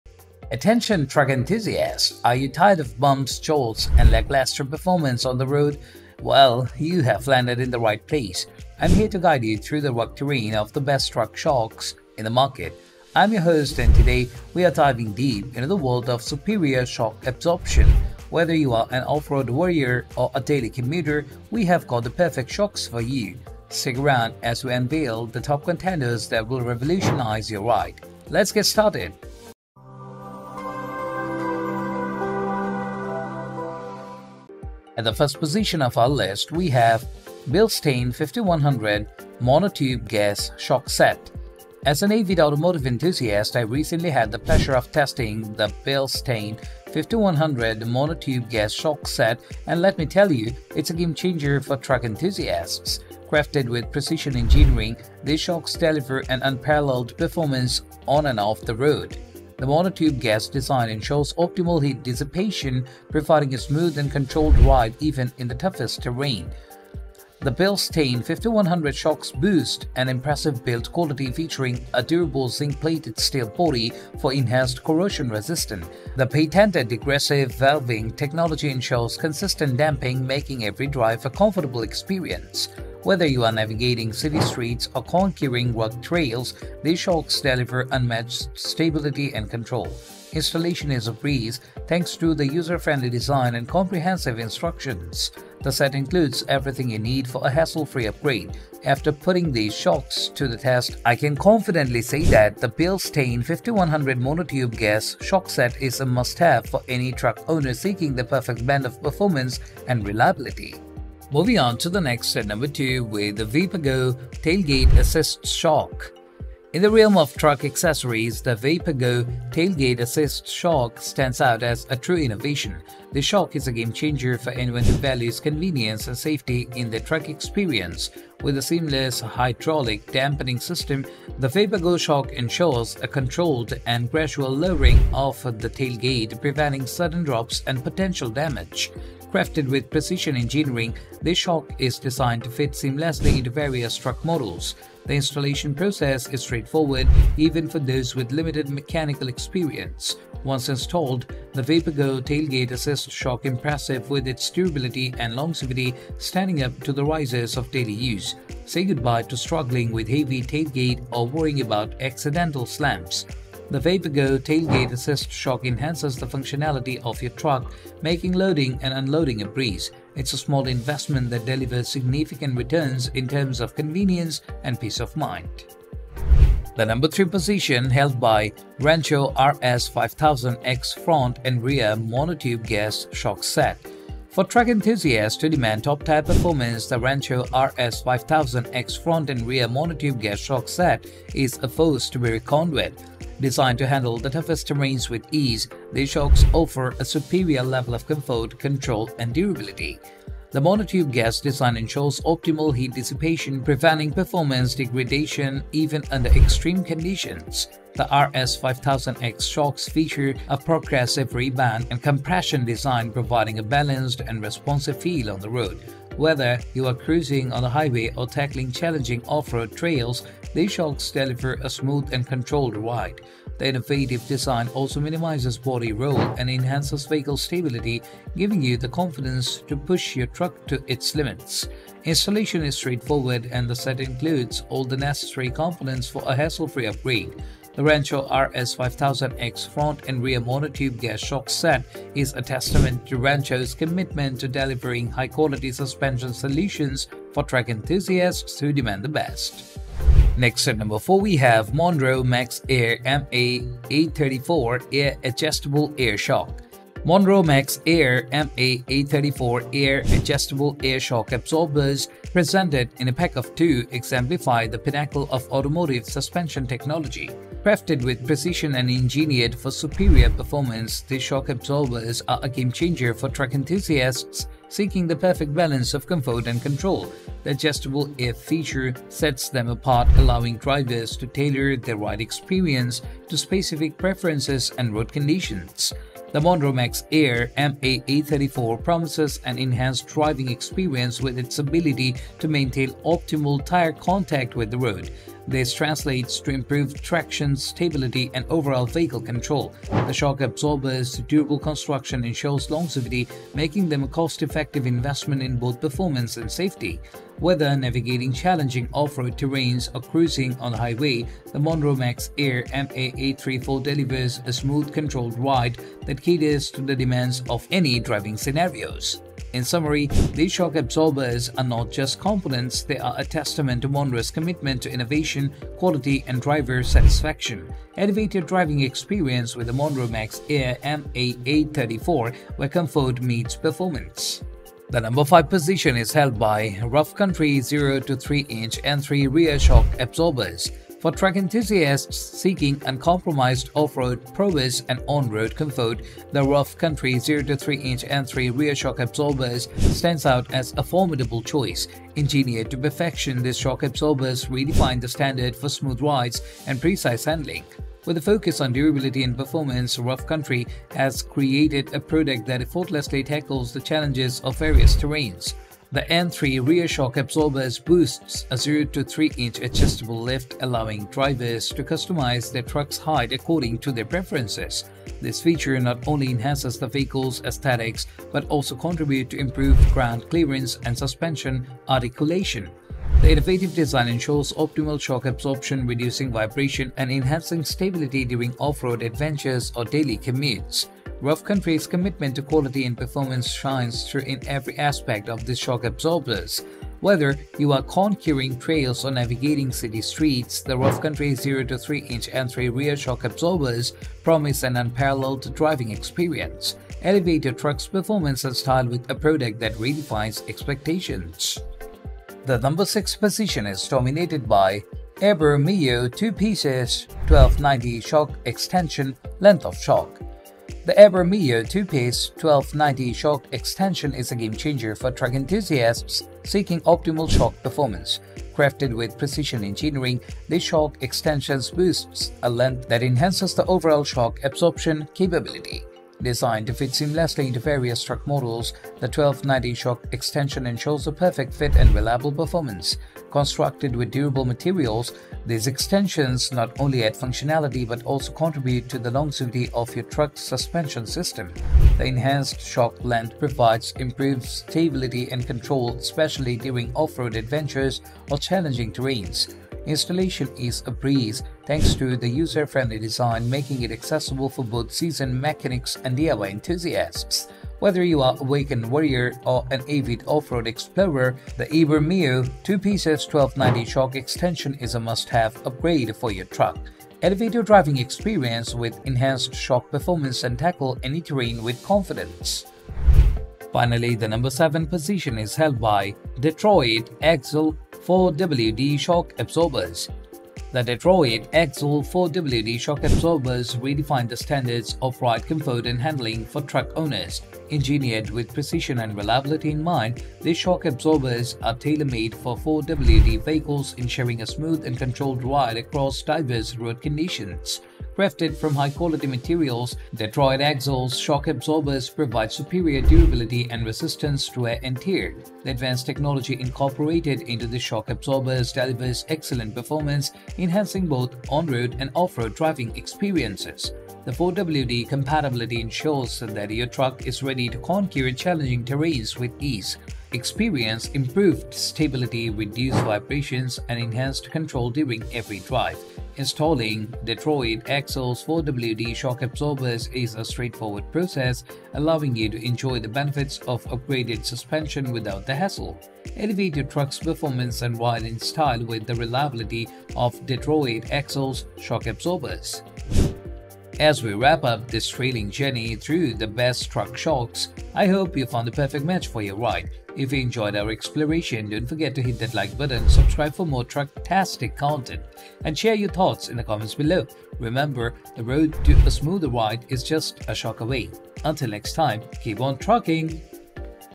Your Truck Deserves the Best: Top 7 Shocks for Peak Performance
Top 7 Best Truck Shocks in 2024

Links to the Best Truck Shocks we listed in today's Truck Shock review video:

1 Bilstein 5100 Monotube Gas Shock Set

2 Vepagoo Tailgate Assist Shock (?43301)

3 Rancho RS5000X Front and Rear Monotube Gas Shocks Set

4 Monroe Max-Air MA834 Air Adjustable Air Shock Absorber Pack of 2

5 Rough Country 0-3" N3 Rear Shock Absorbers

6 Ebrmeiwo 2 Pcs 1290 Shock Extension, Length of Shock

7 Detroit Axle - 4WD Shock Absorbers

What are the Best Truck Shocks in 2024?

In today's video we reviewed the Top 7 Best Truck Shocks on the market in 2024. We made this list based on our personal opinion and we ranked them in no particular order, after doing our research based on their prices, quality, durability, brand reputation and many more.

After watching this video you will know which are the best budget, top selling and top rated Truck Shock on the market today.

If you choose from this list you can be sure that you buy one of the best Truck Shocks available today.